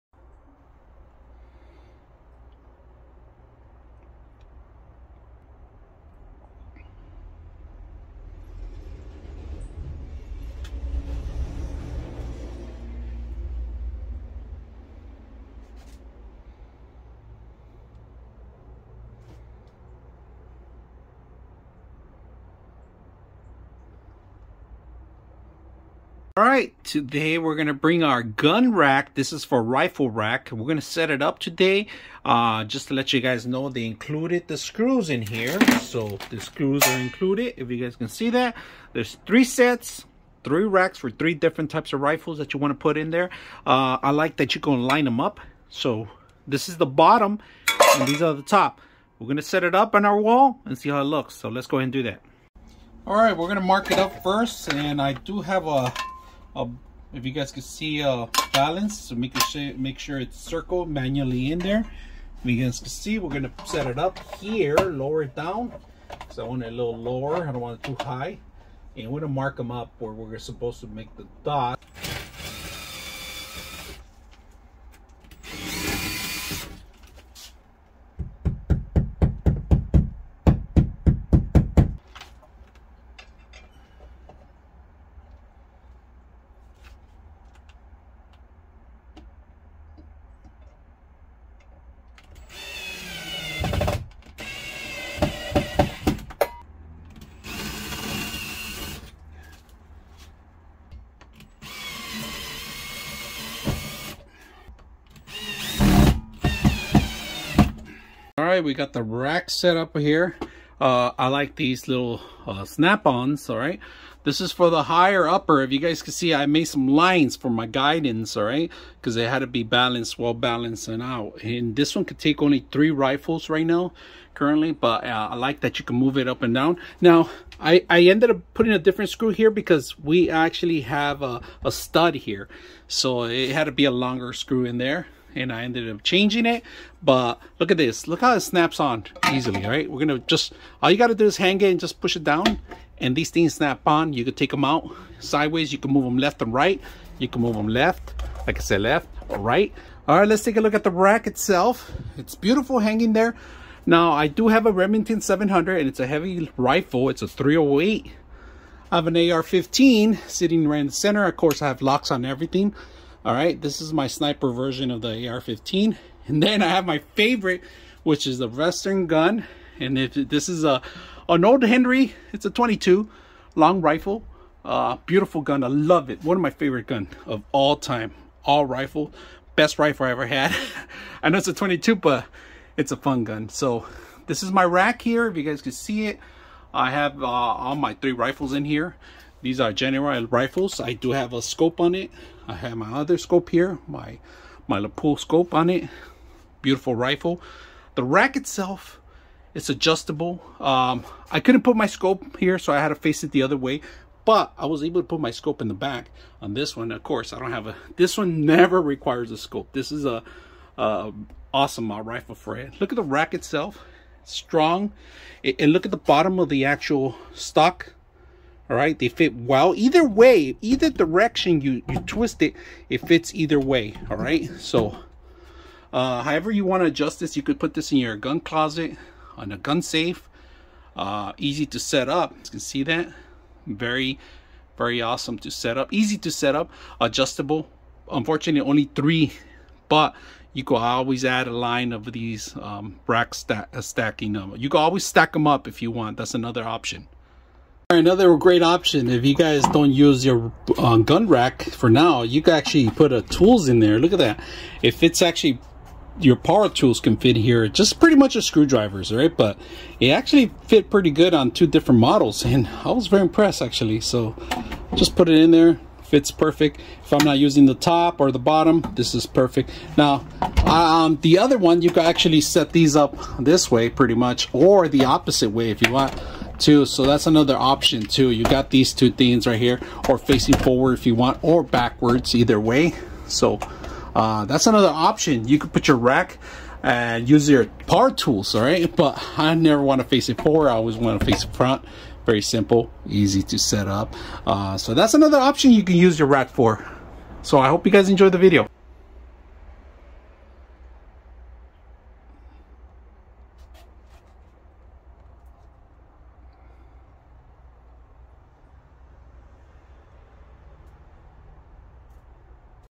Gun Rack
Nonkky Gun Rack, Adjustable Gun Racks for Wall, Heavy Duty Steel Indoor Gun Racks Holds up to 150 Lbs, Rifle Rack Display Stand with Soft Padding